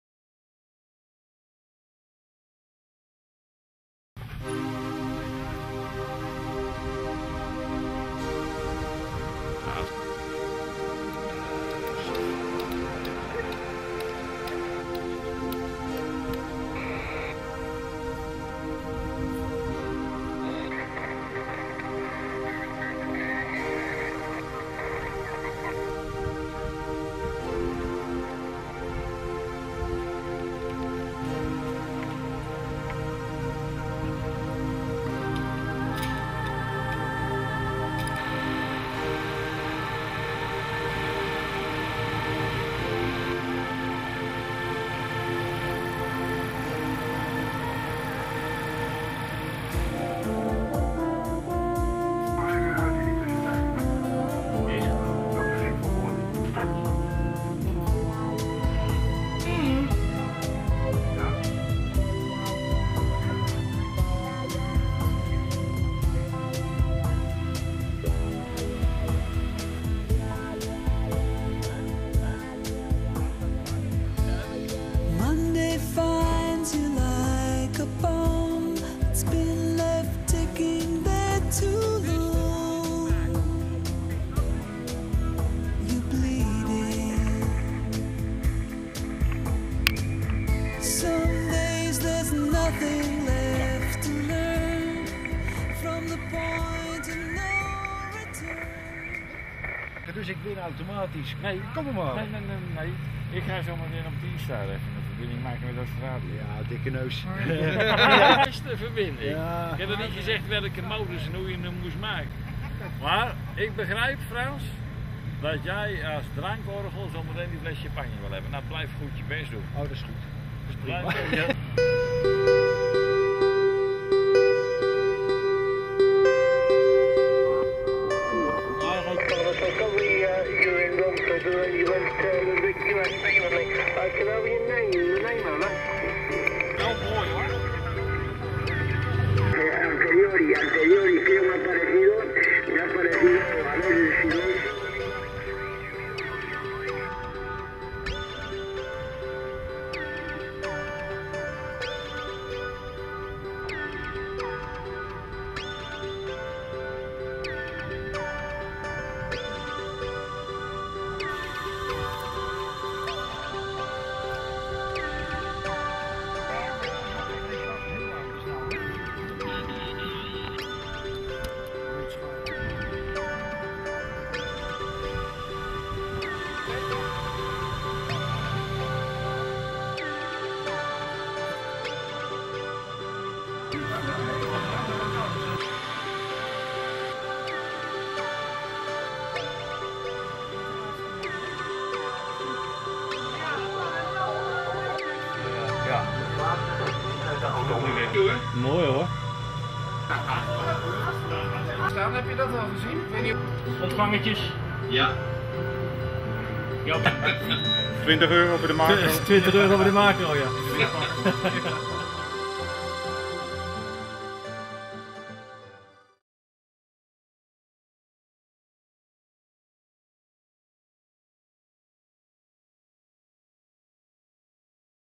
Black Trousers HAM Radio Event
Het jaarlijkse HAM Radio velddag gebeuren in Zwartebroek